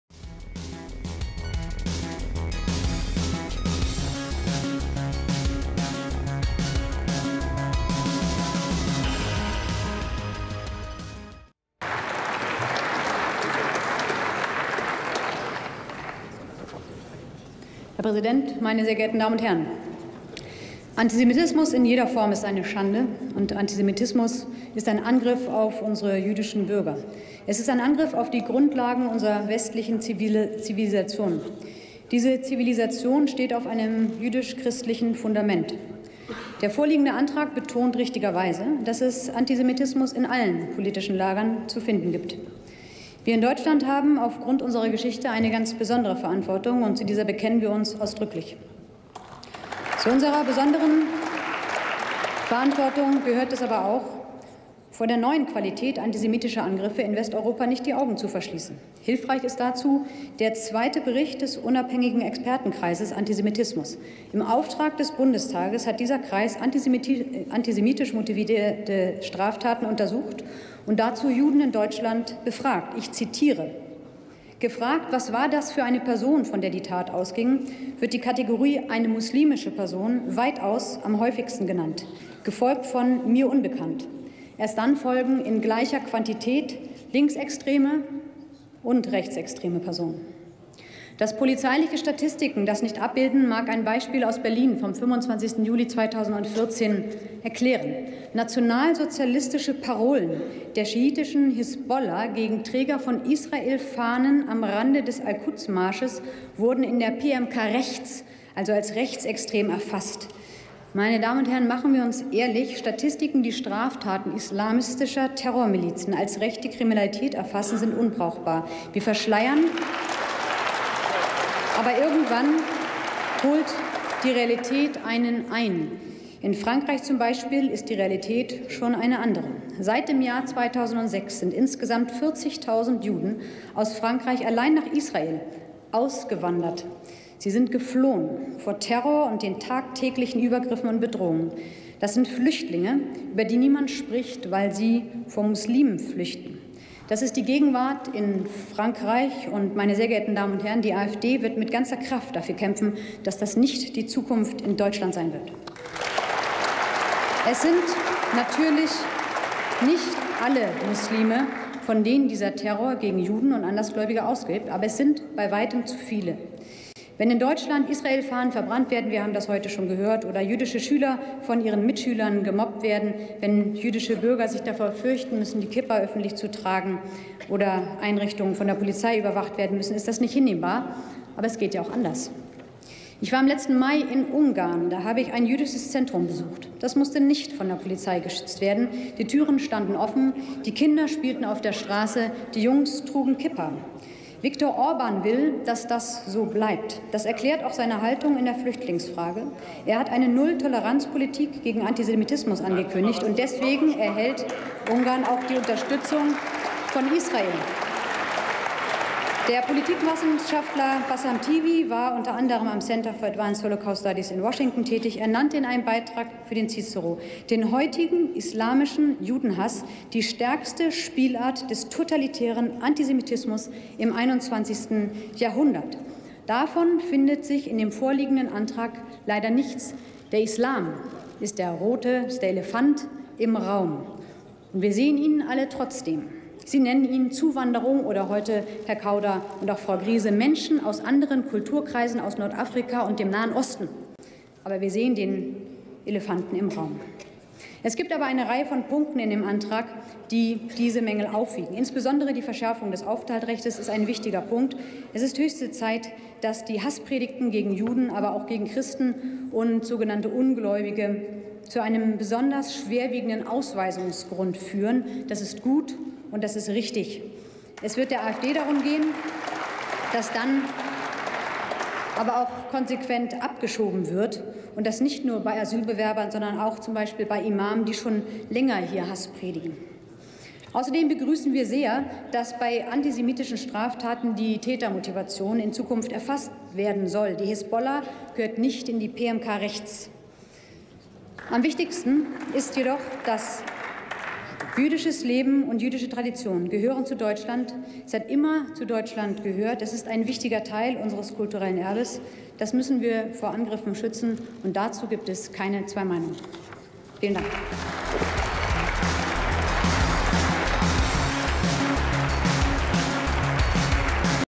ANTISEMITISMUS wird IMPORTIERT! Beatrix von STORCH zeigt Inkonsistenz der Altparteien * AfD im BT
Beatrix von Storch zeigt die Inkonsistenz der Altparteien beim Antisemitismus auf: Die islamische Zuwanderung ist importierter Antisemitismus. 26.01.18